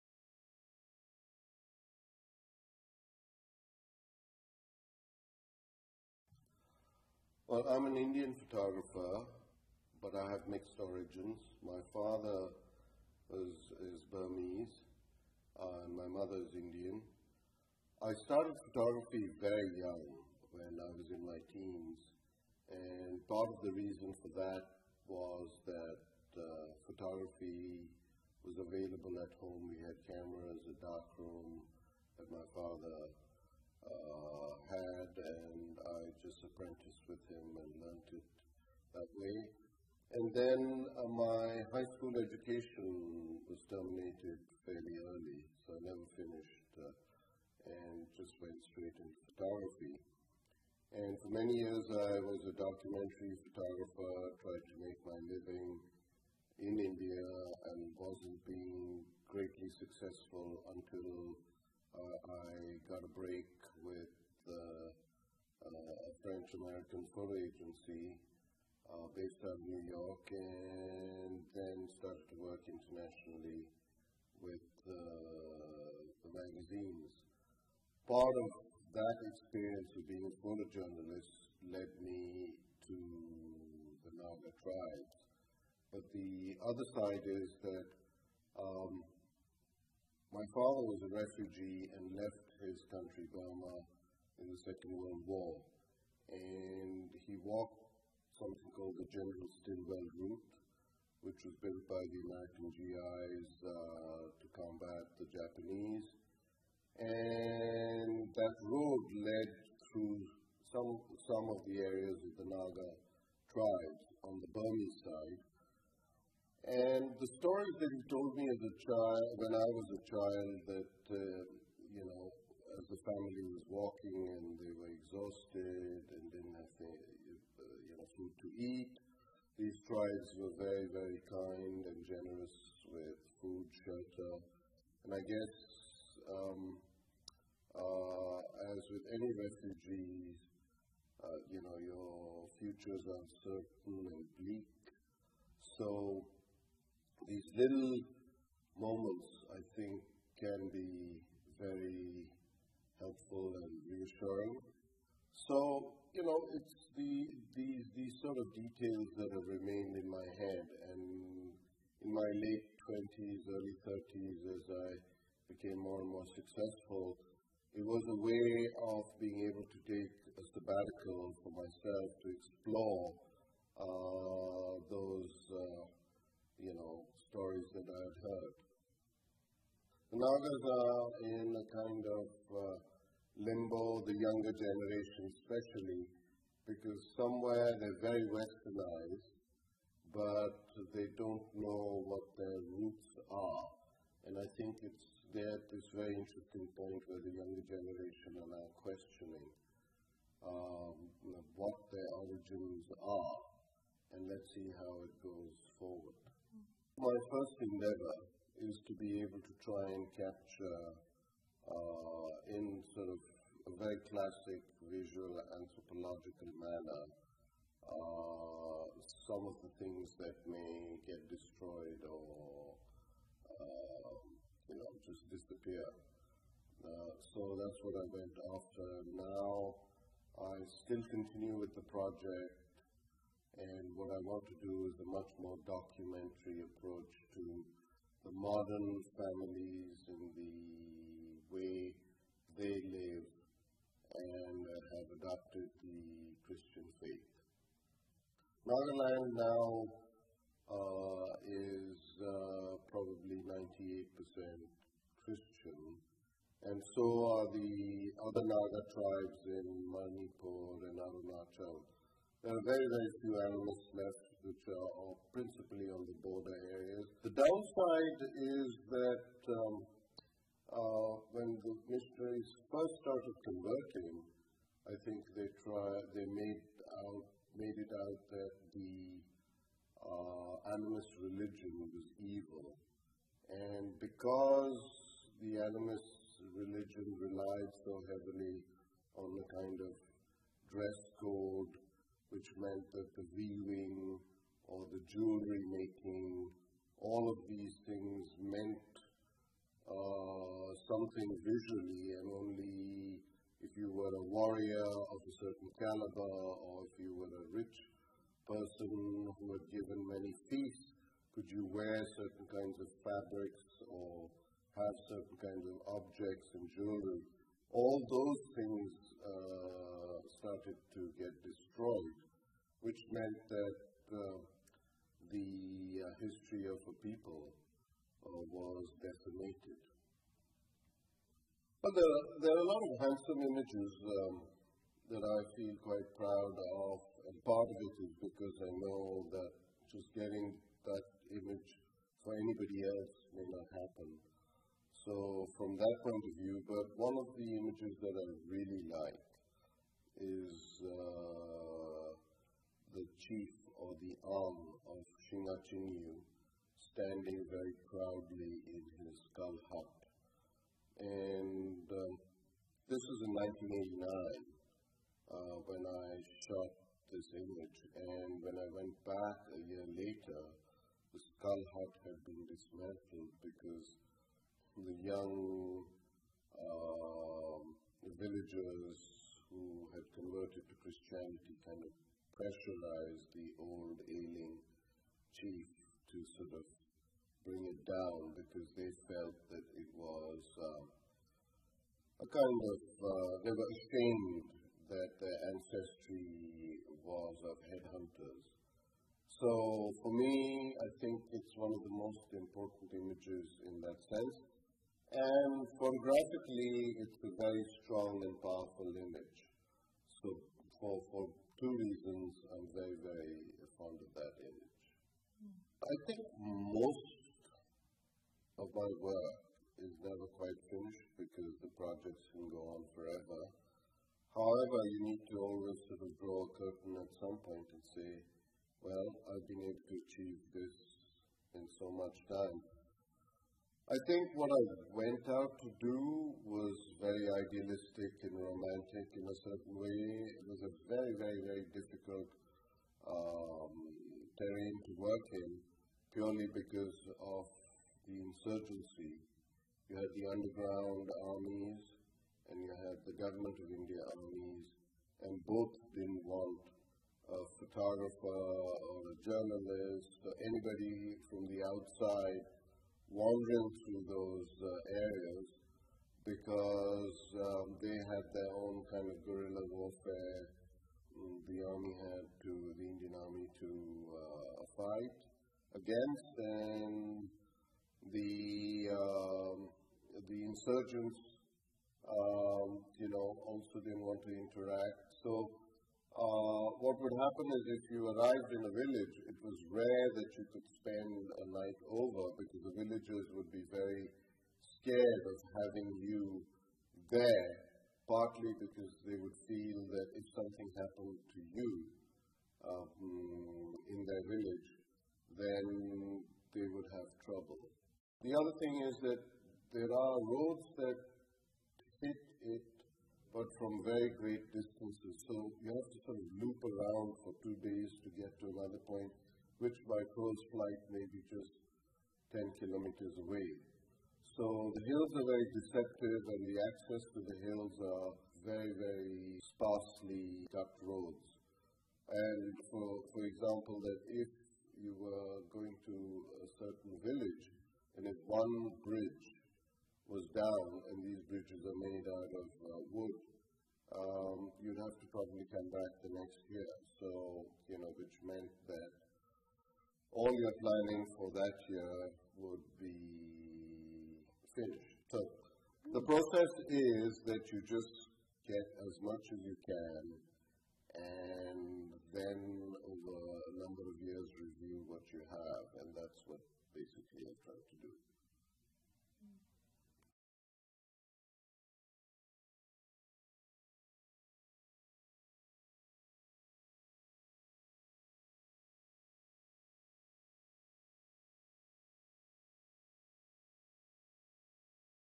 Interview with Pablo Bartholomew
December 2009 interview with photographer Pablo Bartholomew. Produced in conjunction with the exhibition Nagas: Hidden Hill People of India on view in the Theater Level gallery March 13, 2009 - September 21, 2009.